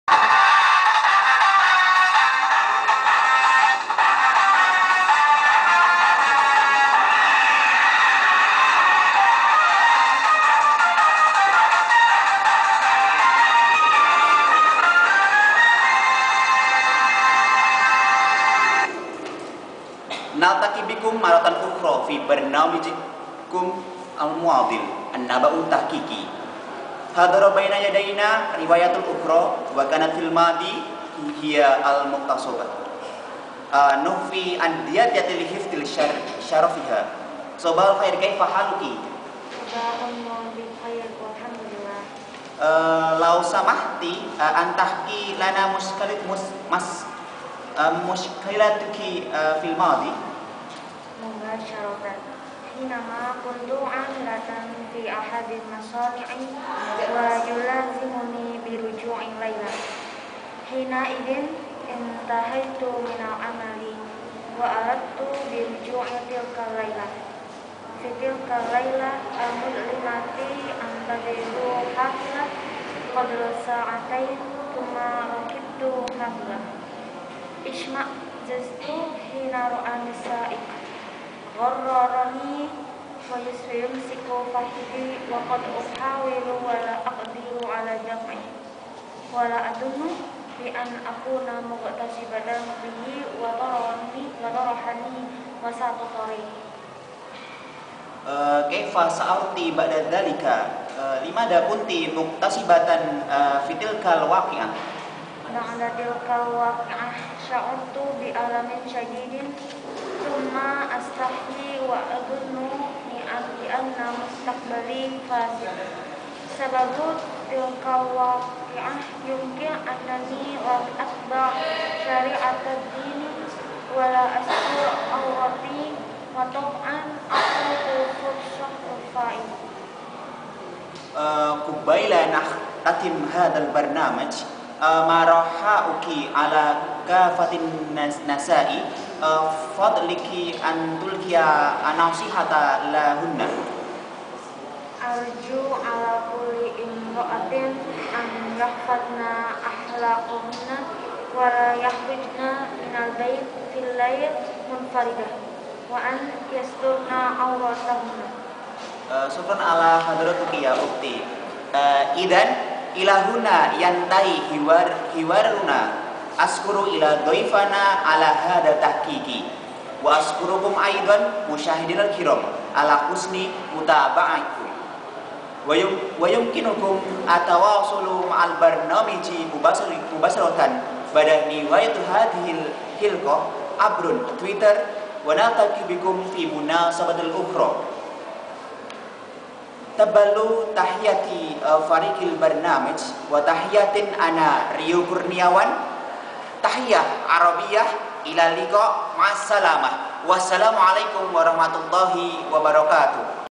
Berita Investigasi Arabic Program Live in PBA-TV (Bahasa dan Sastra Arab IAIN Surakarta) Part 3
Video di Atas Berita Investigasi Arabic Program (Bahasa dan Sastra Arab IAIN Surakarta), dengan Judul Maraknya Pemerkosaan dikarenakan Rendahnya Pendidikan dan Rusaknya Moral, di Tayangkan langsung oleh PBA-TV, Kritik dan Saran kami harapkan, dan Selamat menyaksikan: